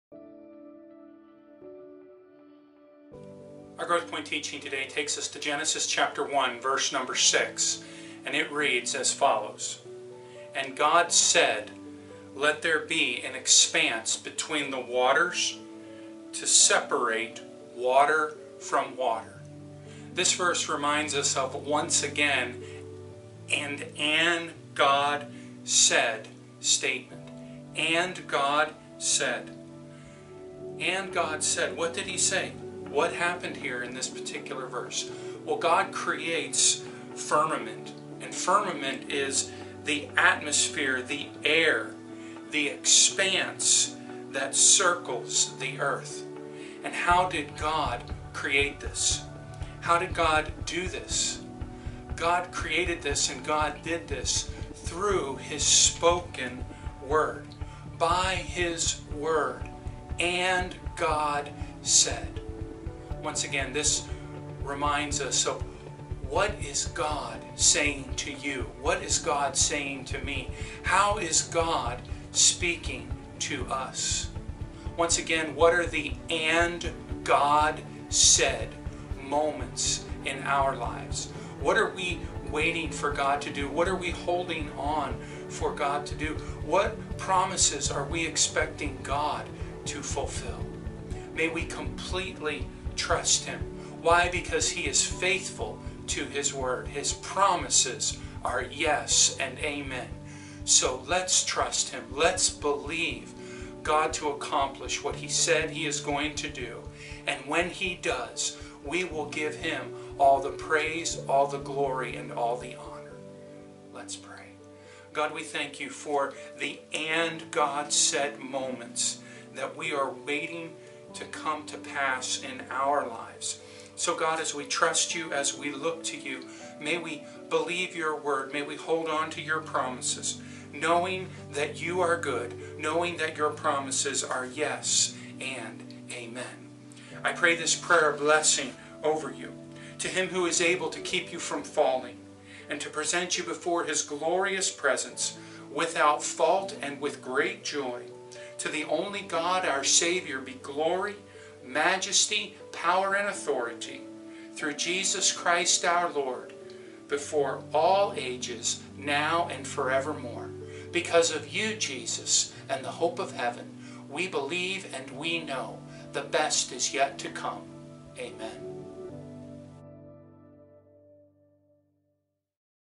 Genesis 1:6, 052521
Today's GrowthPoints teaching reflects on thoughts from Genesis 1:6. Join Pastor Chris Light as he shares from the WORD of God.